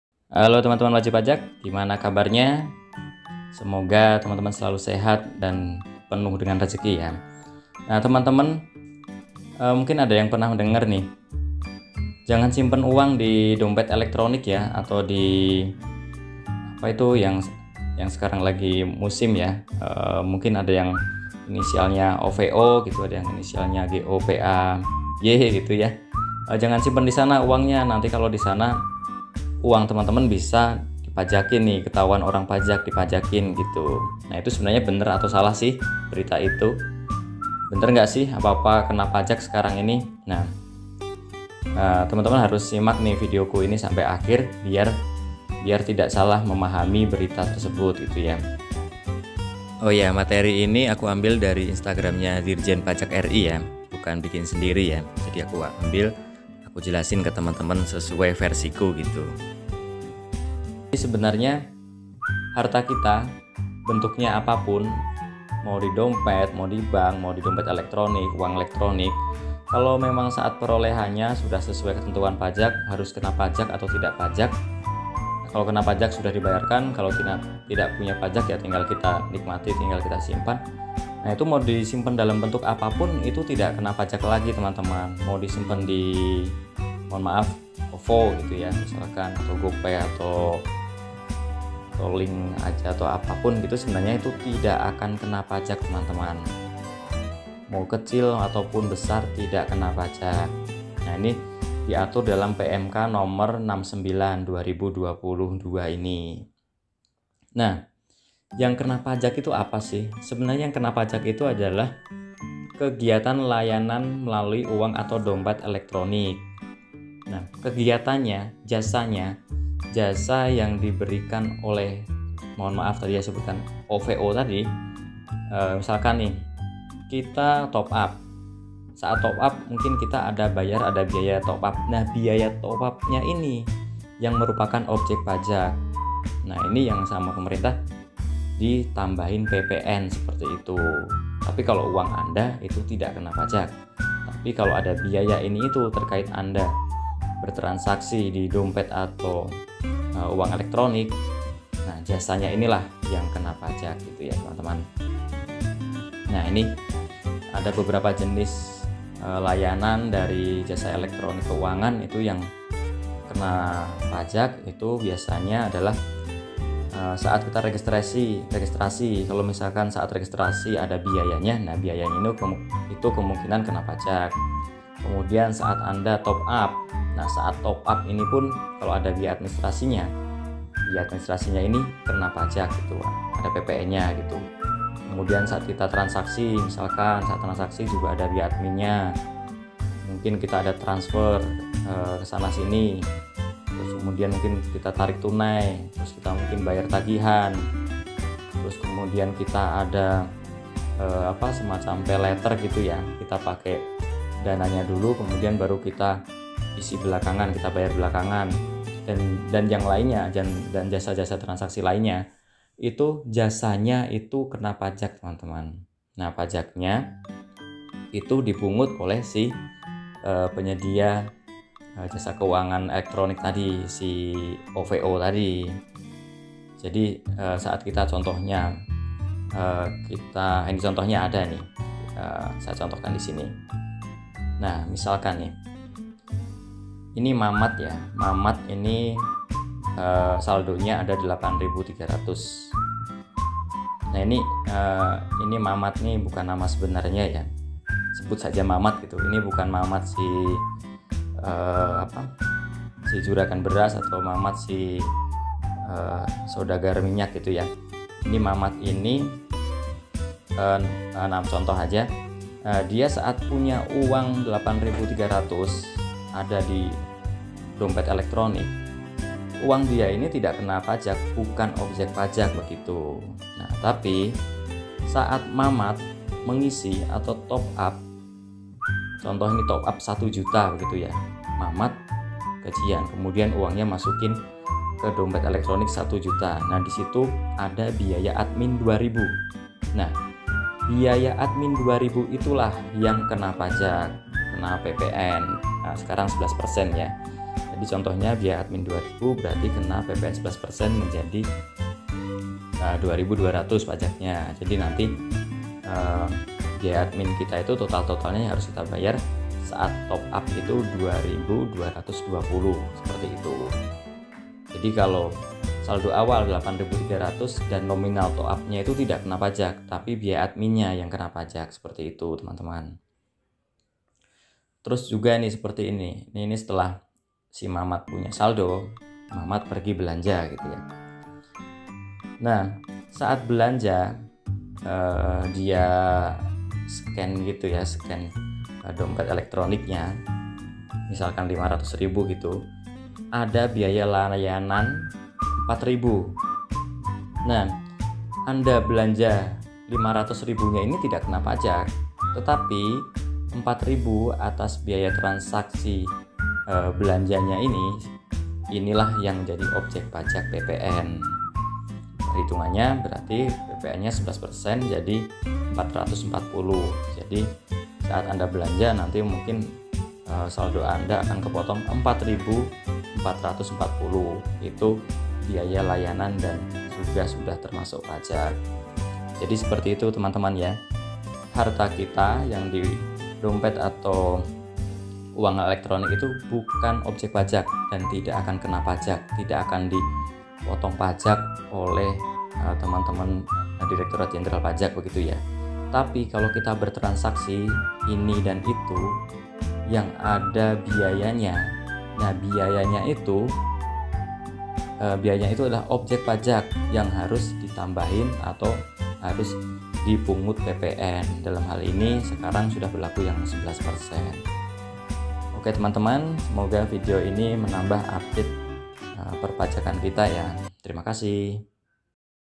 UANG ELEKTRONIK APAKAH KENA PAJAK? 69/PMK.03/2022
Layanan Jasa Perpajakan

Teman-teman ada yang mempunyai Uang atau dompet elektronik, dan sedang bertanya-tanya atau sedang ketakutan uangnya akan di potong pajak? Video saya ini menjelaskan kalau uang/dompet elektronik itu tidak kena pajak, tetapi ada aspek lain yang menjadi objek pajak terkait aktivitas uang/dompet elektronik ini.

Untuk lebih lengkapnya teman-teman bisa baca peraturannya di 69/PMK.03/2022 yang dapat di download di link ini ya :